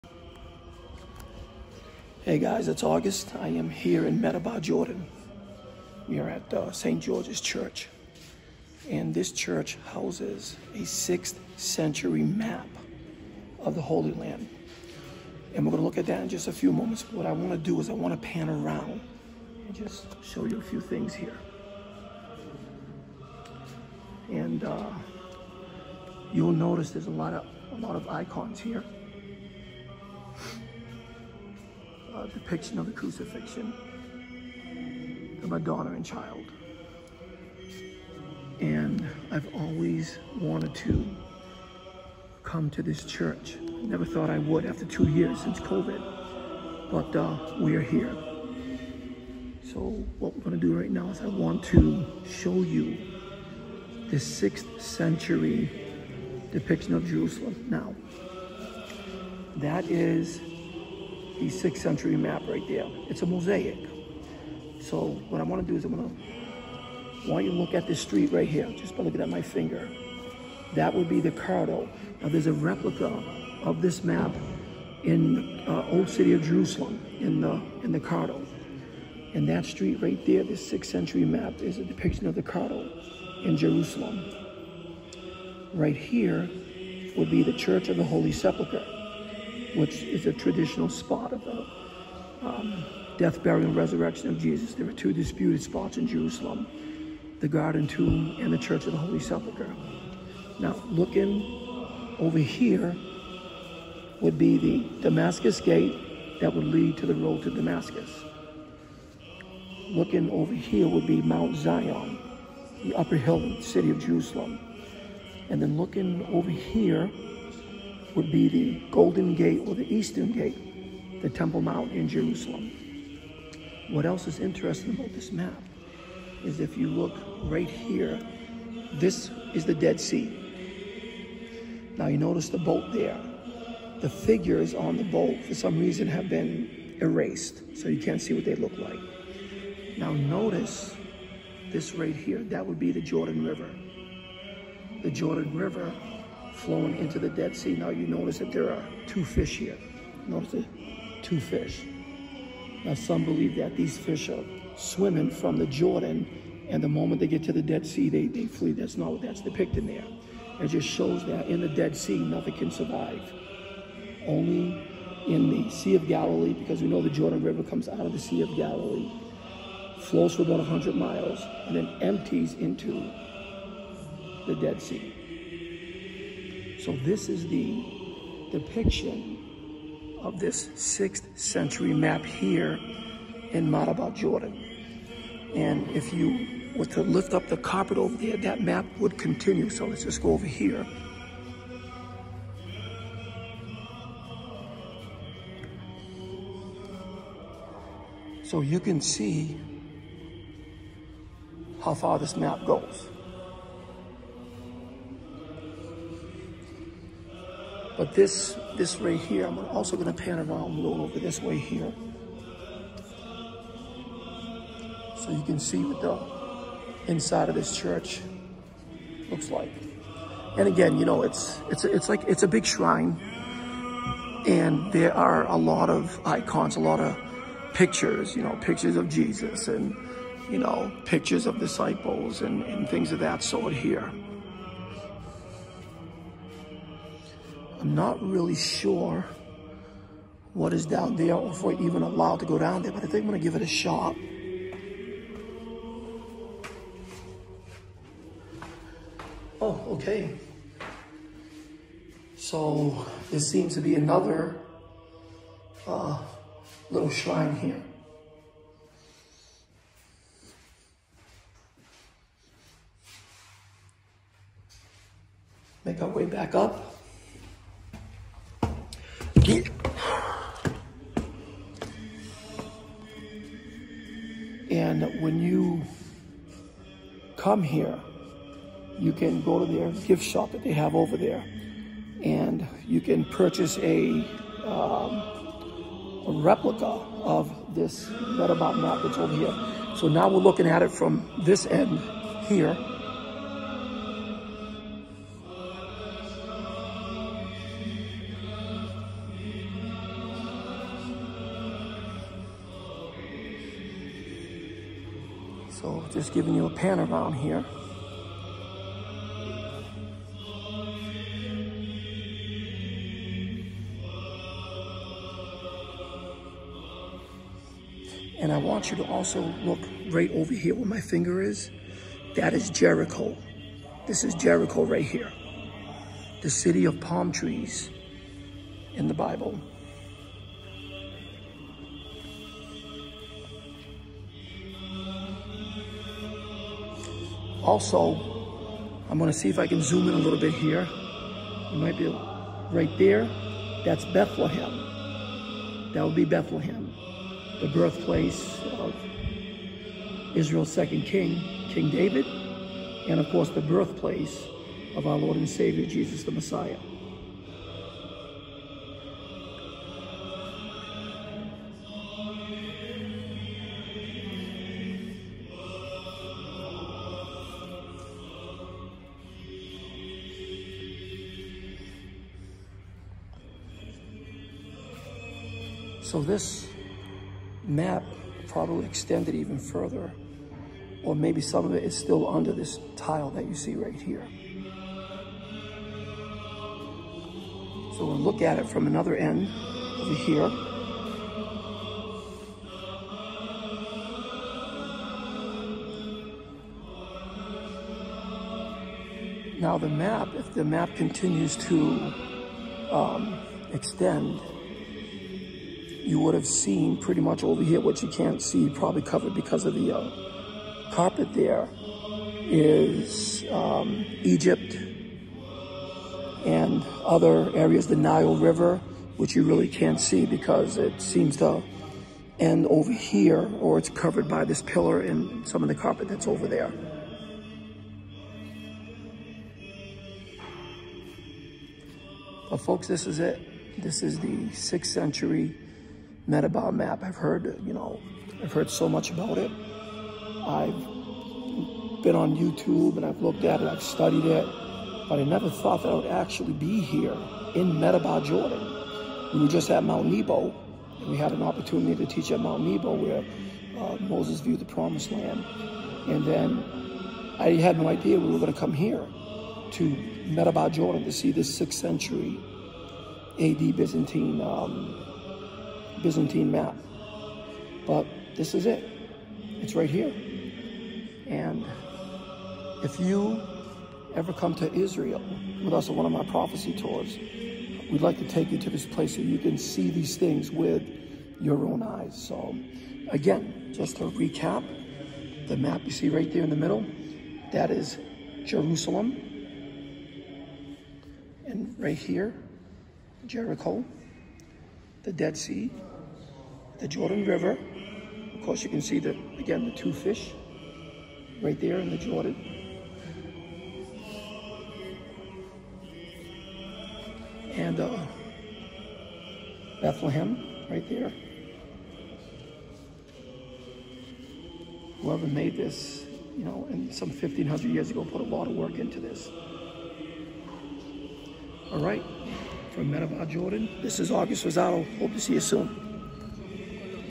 The Medaba Map in Jordan
Dr. August Rosado is at St. George Church in Medaba Jordan explaining the 6th century Medeba map depicting Jerusalem.